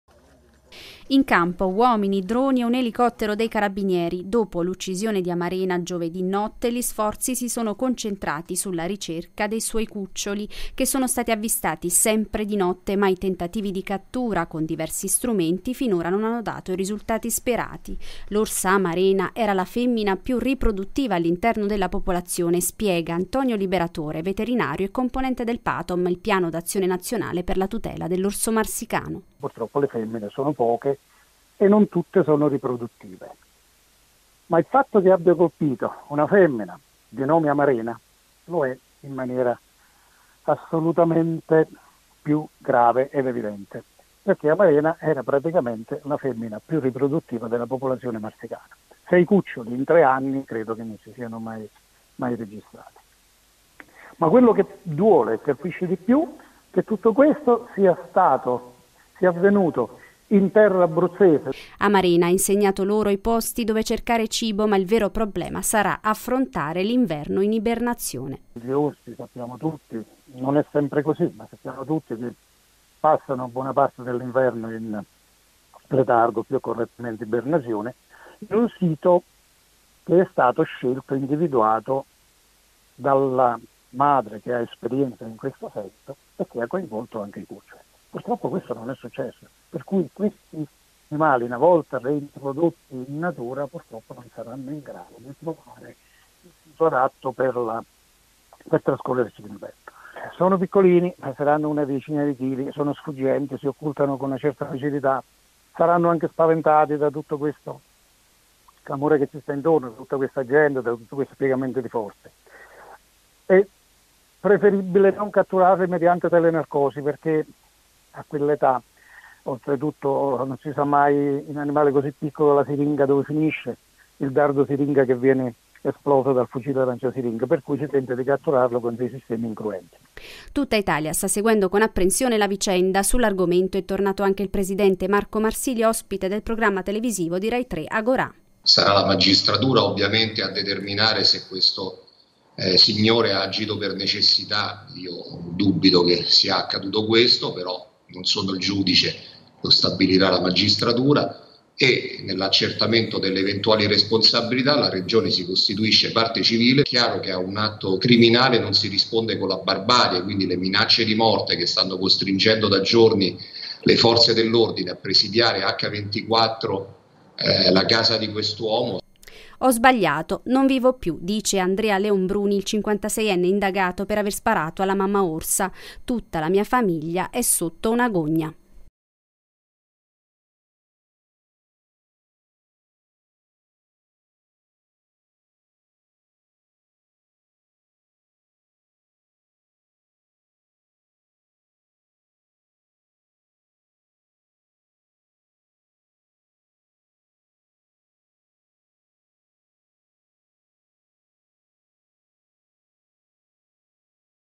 Affrontare l'inverno, per l'esperto è la vera sfida per i cuccioli di Amarena -...
Servizi TG | Affrontare l'inverno, per l'esperto è la vera sfida per i cuccioli di Amarena | 4/9/2023